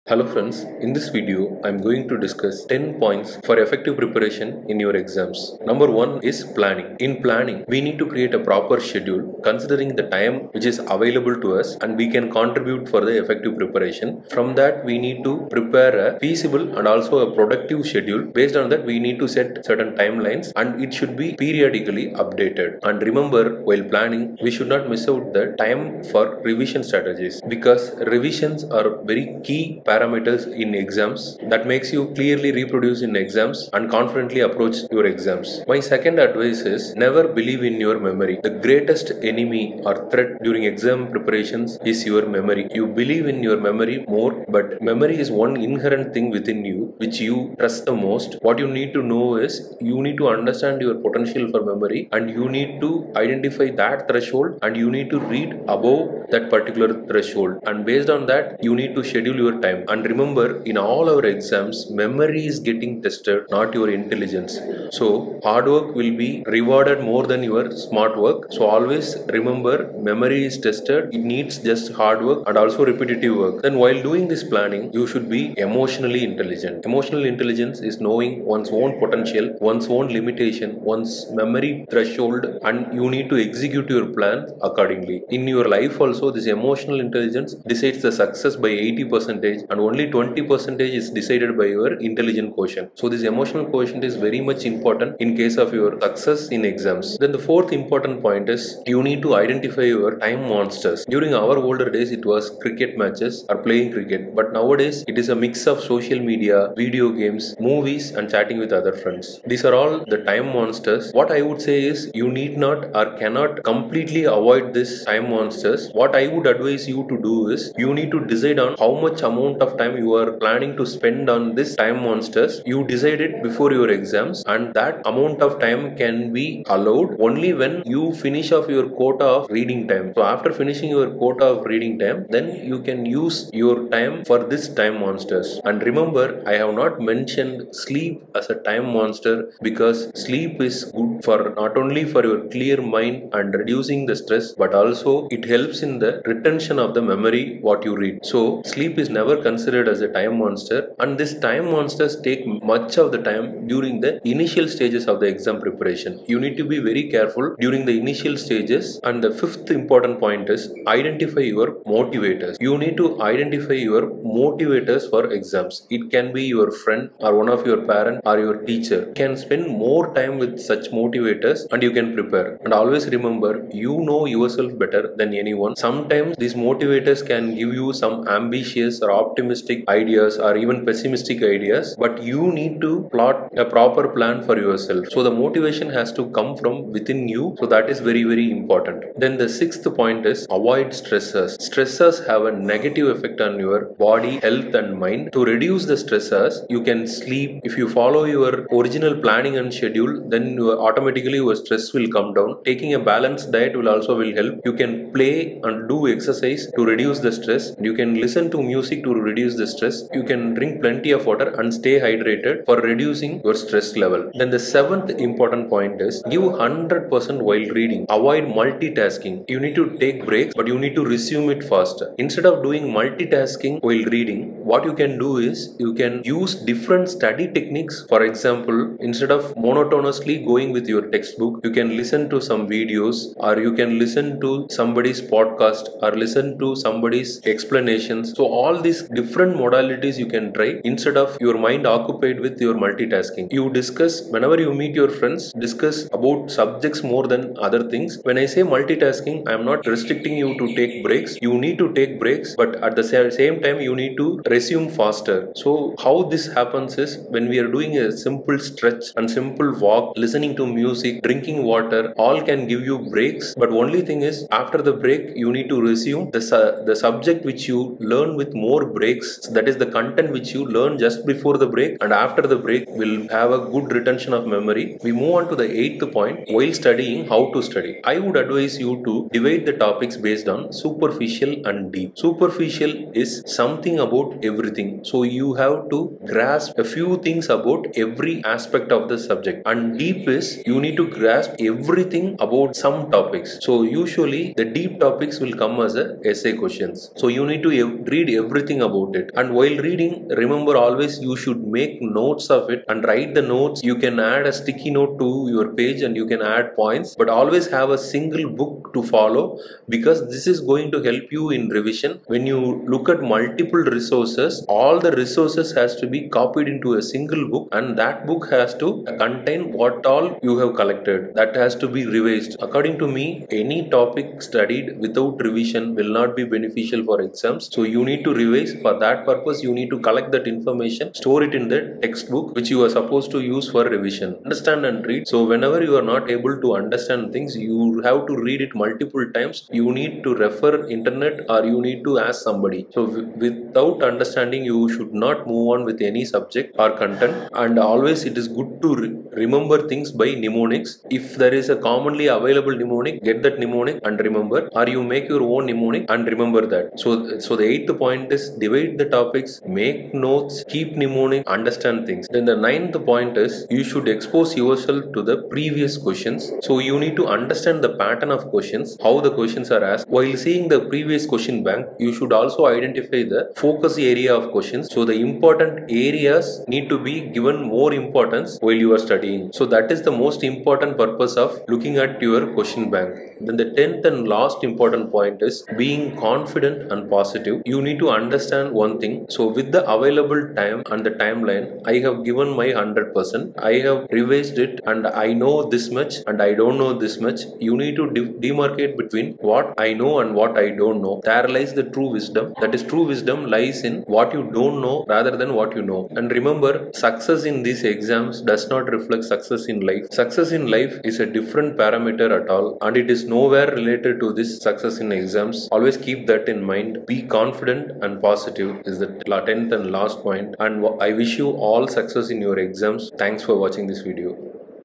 10 Tips for success in exams - Dr Rock Britto
Deals with 10 simple Tips to students for success in exams.
1. Planning – Schedule, Timeline and Revision
2. Never believe in your memory, But exam test memory.
3. Be Emotionally intelligent - Understand yourself.
4. Identify the time monsters
5. Identify your motivators
6. Reduce stress – Sleep, diet, hydration, play
7. Avoid multitasking, go multimodal, Take breaks and resume
8. Divide into Superficial and deep, Make notes, Understand, and Make mnemonics.
9. Look at Previous questions - Understand the pattern and focus areas.
10. Being confident and positive - Success in exams ≠ Success in Life .